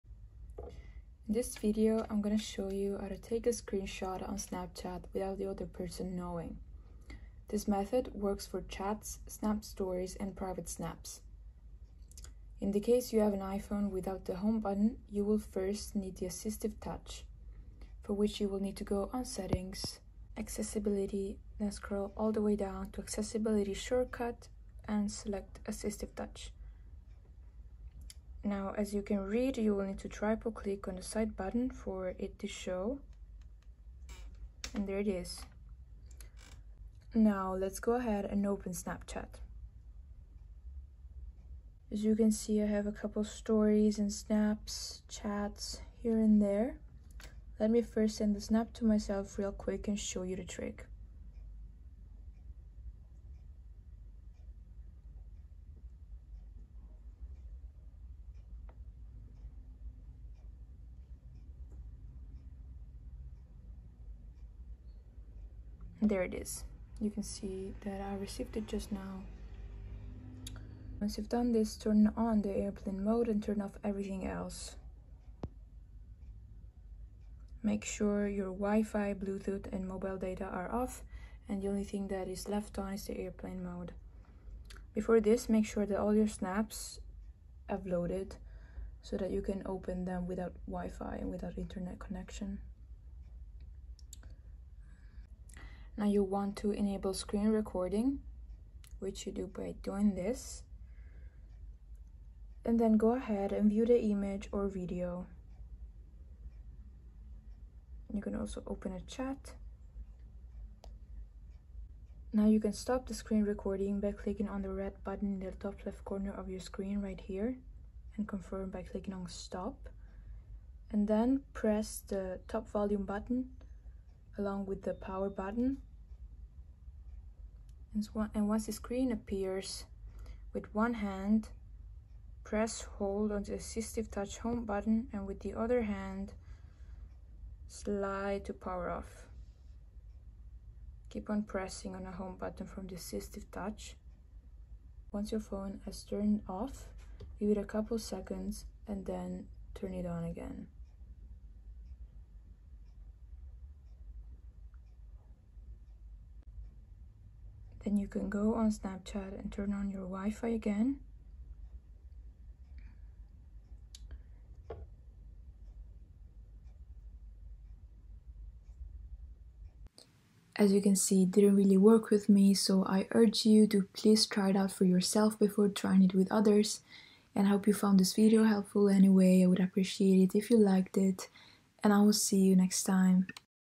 How To Take A Screenshot On Snapchat Without It Showing
Learn How To Take A Screenshot On Snapchat Without It Showing

In this video I will show you how to take screenshot on Snapchat without it showing in 2021. This will be a short guide that shows you exactly how to take a screenshot on Snapchat without notification.